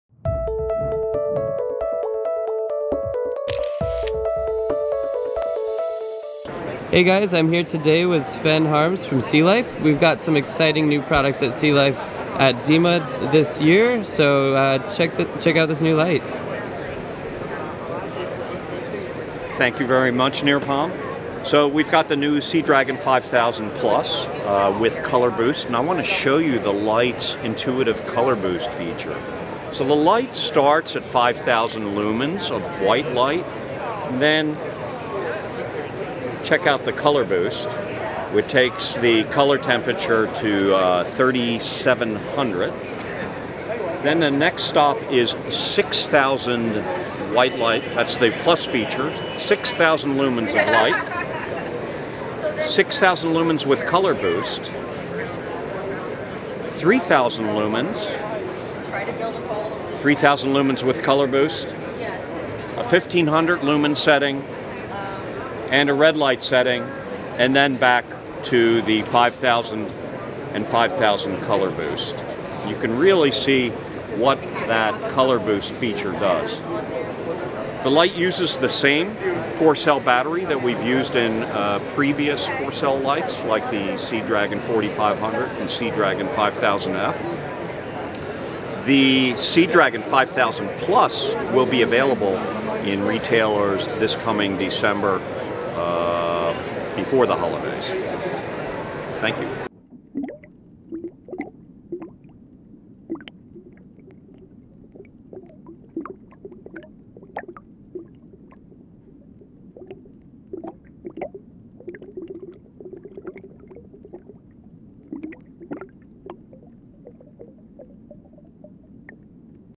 The New SeaLife Sea Dragon 5000+ | DEMA 2022
The SeaLife Sea Dragon 5000+ is an exciting new underwater video light debuted at DEMA 2022. It features a 6000 lumen extended lumen mode and a "color boost" mode that brings out reds normally lost underwater. Join us as we talk to Sven Harms from SeaLife about this cool new light!